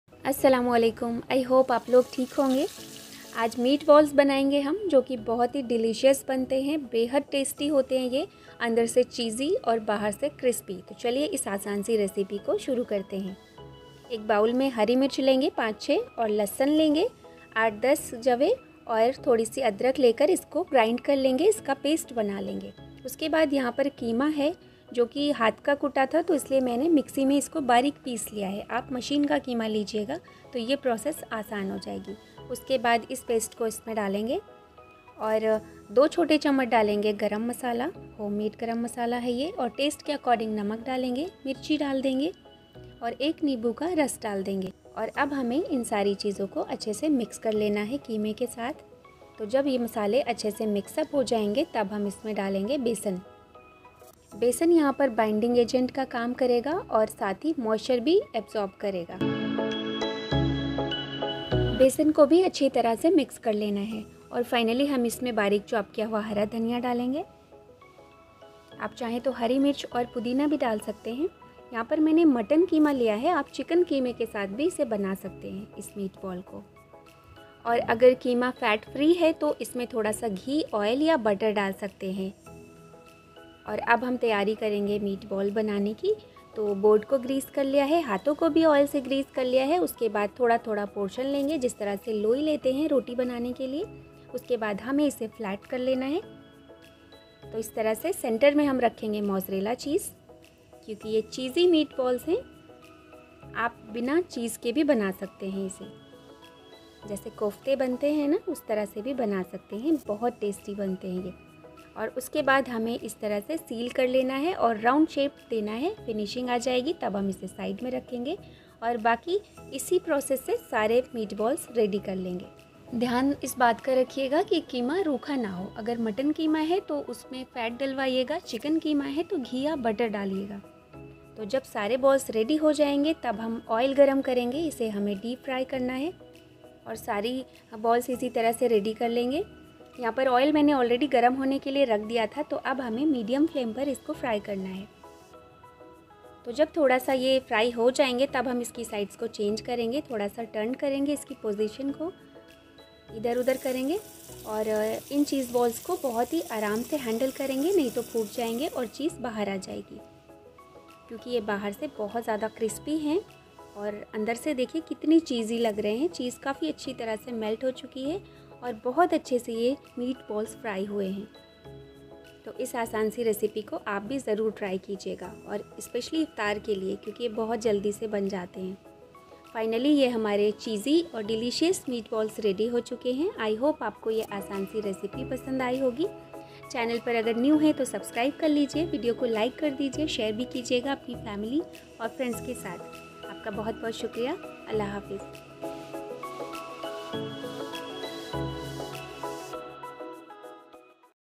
Cheesy & Crispy Meatballs❗Cheese Stuffed Kebab ❗Mutton Kabab❗Iftar Recipes ❗ Non-vegetarian Snacks❗
Meatballs prepared with mutton mince seasoned with some basic spices, turned out ultimate! Sooo cheesy from inside & crispy from outside!! This is the outstanding snack i have found, cheese stuffed meatballs or kofta kabab! Unique & Must try easy recipe!
Your visits likes subscriptions & comments are highly valued 🙏

Check out some more enjoyable delicious and easy recipes 🌷